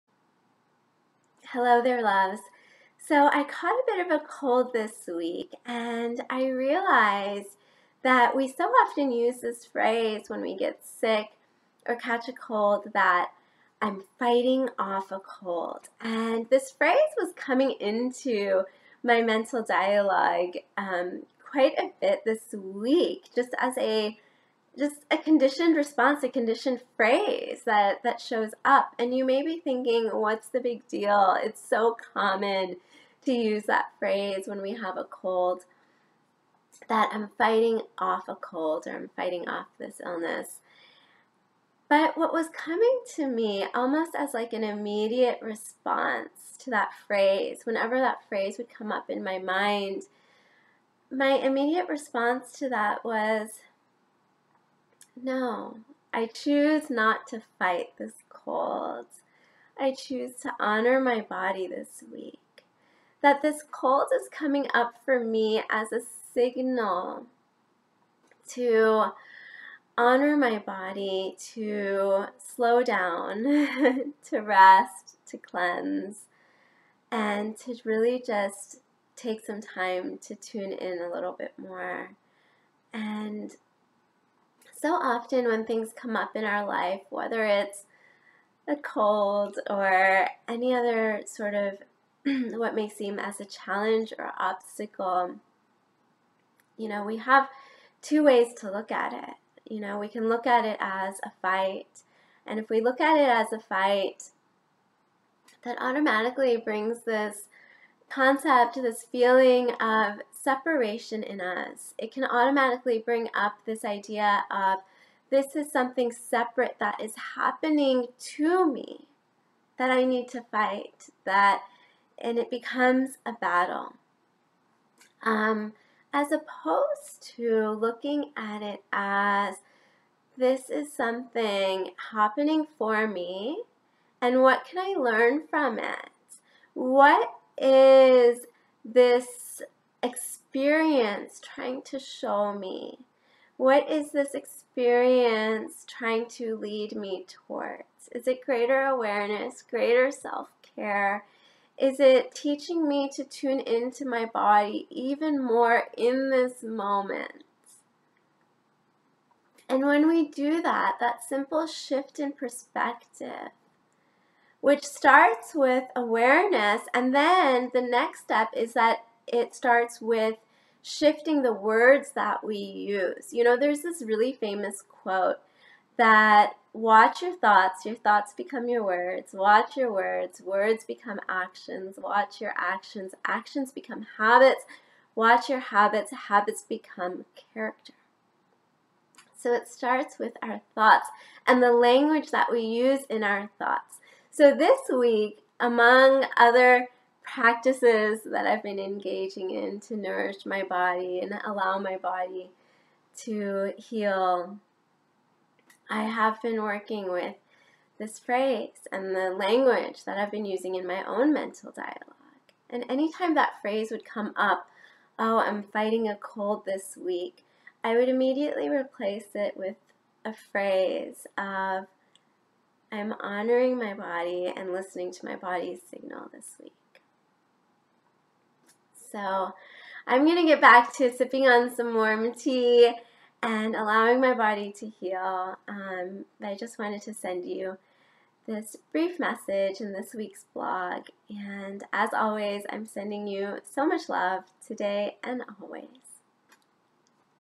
Honoring our Obstacles
We always have a choice.  We can view life's challenges as something to fight against, or as something to work with and honor.  In this video, learn how to practice this subtle, yet powerful, shift in perspective!